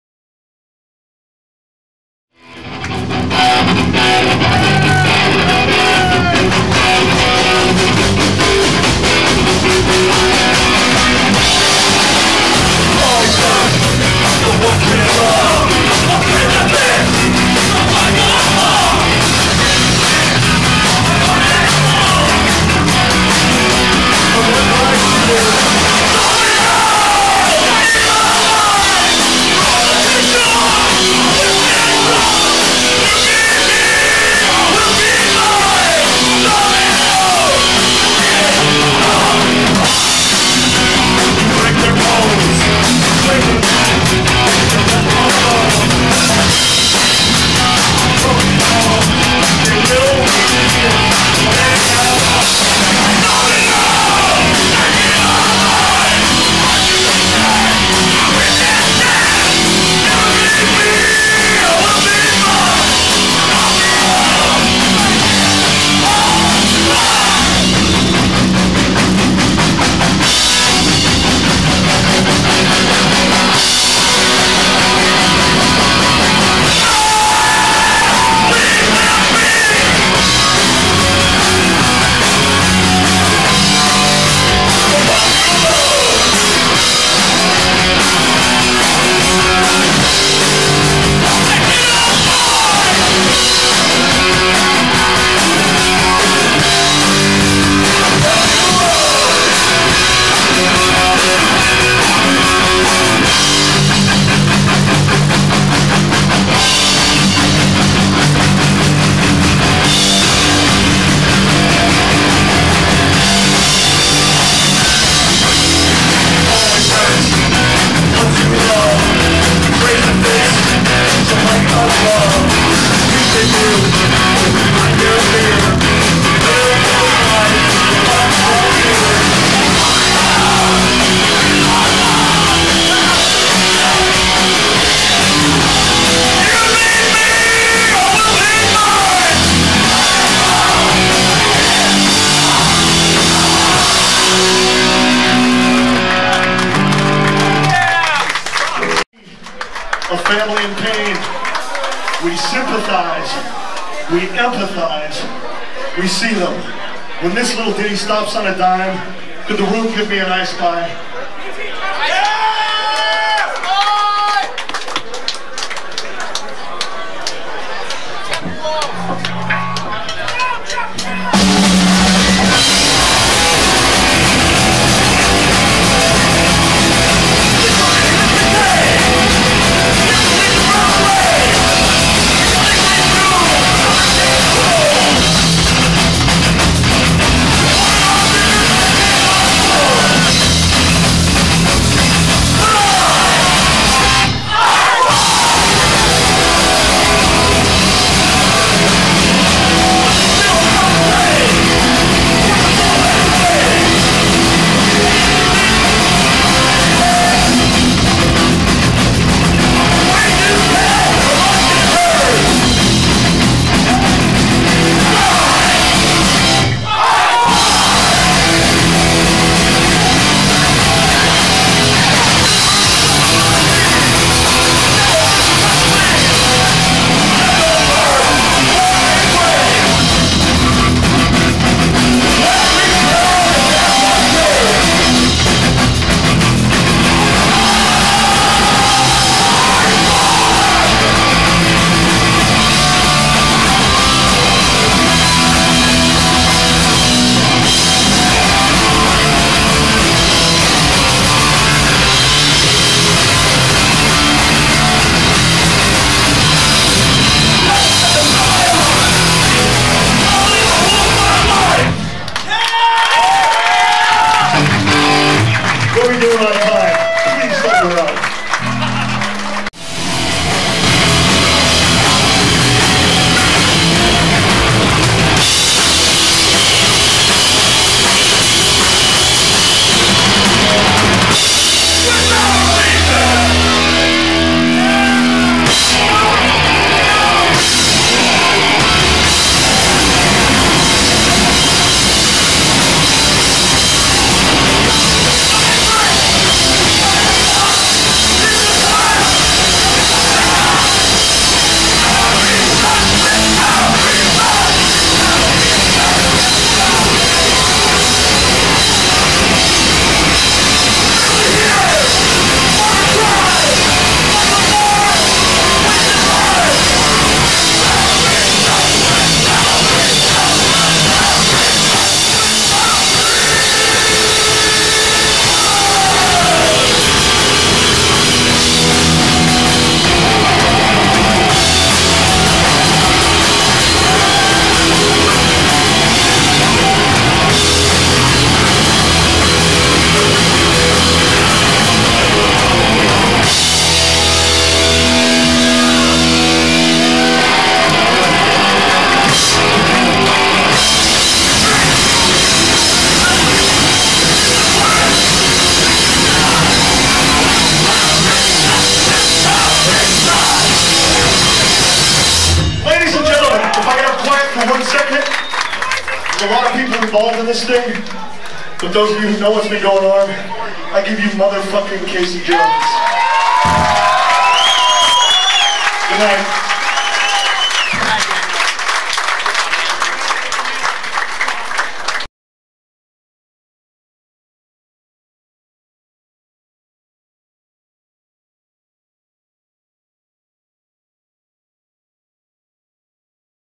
NO FOR AN ANSWER Yost Theater.
NO FOR AN ANSWER reunion for the Jon Bunch Benefit Show at the Yost Theater. Video: Joe D. Foster